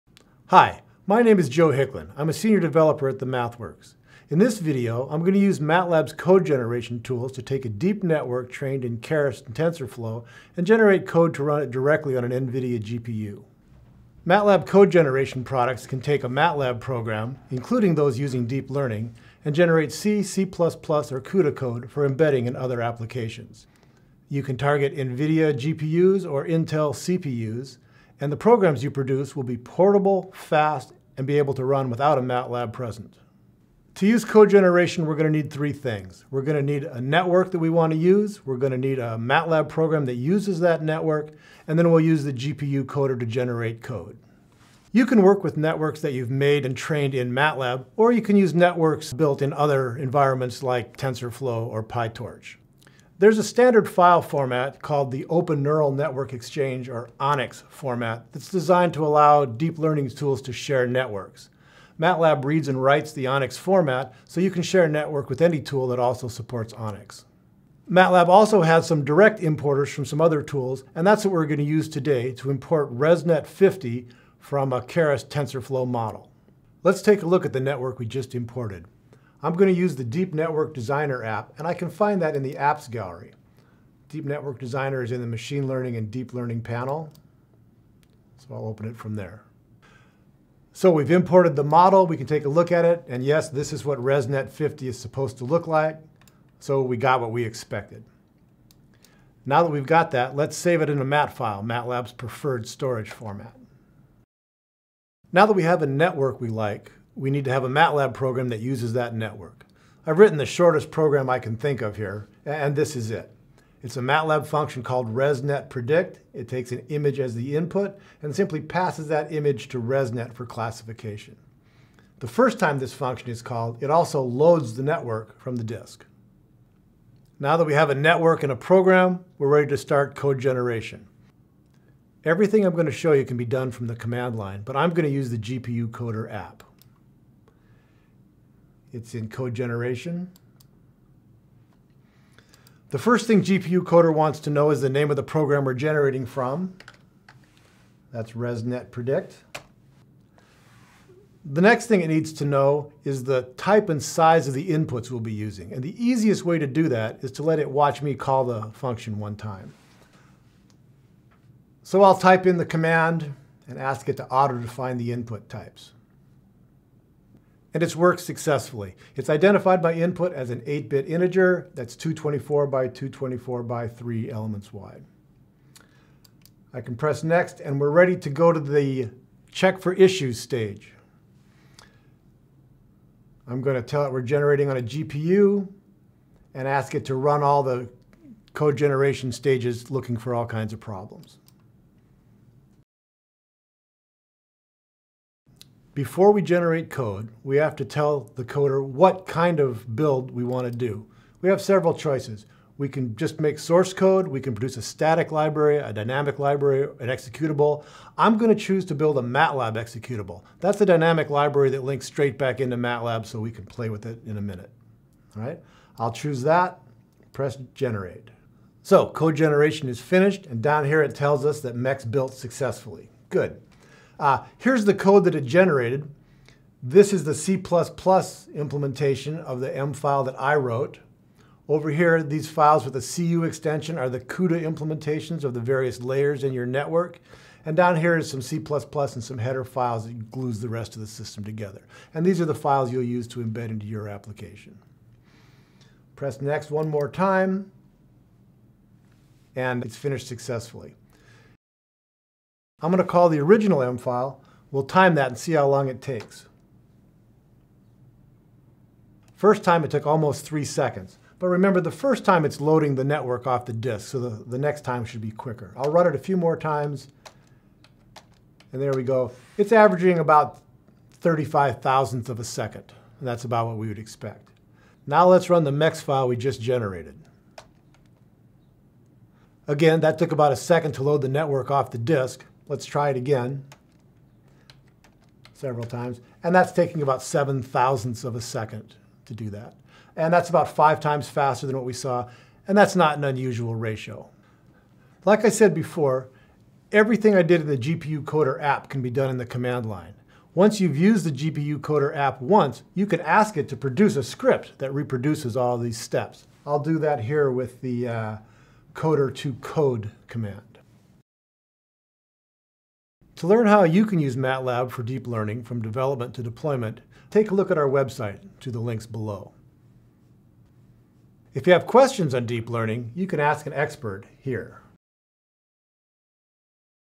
How to Generate CUDA Code for a Keras-TensorFlow Model
This video walks step-by-step through the process of taking a deep network trained in Keras and Tensorflow and generating code to run directly on a GPU.

You will learn how to use MATLAB® code generation tools in order to automatically generate C/C++ and CUDA code from your MATLAB program, so you can embed and run it in NVIDIA® GPUs or Intel® CPUs.